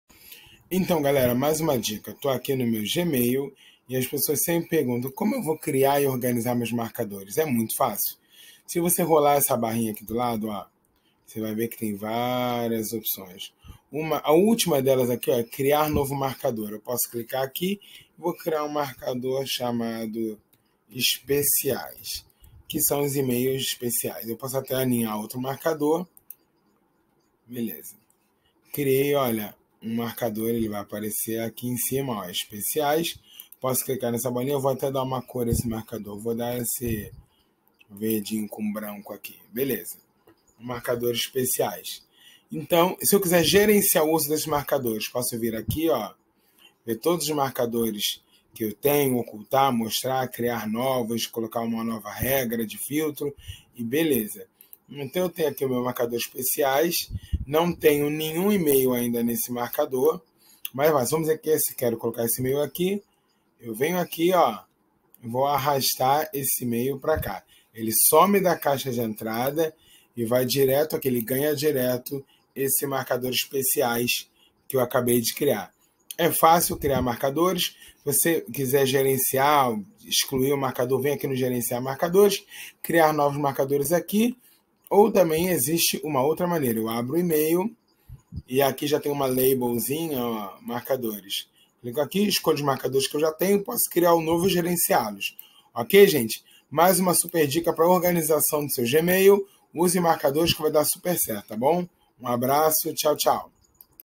Saiba como criar marcadores no Gmail
Na DicaDoDoug você vai aprender como criar marcadores em seu Gmail.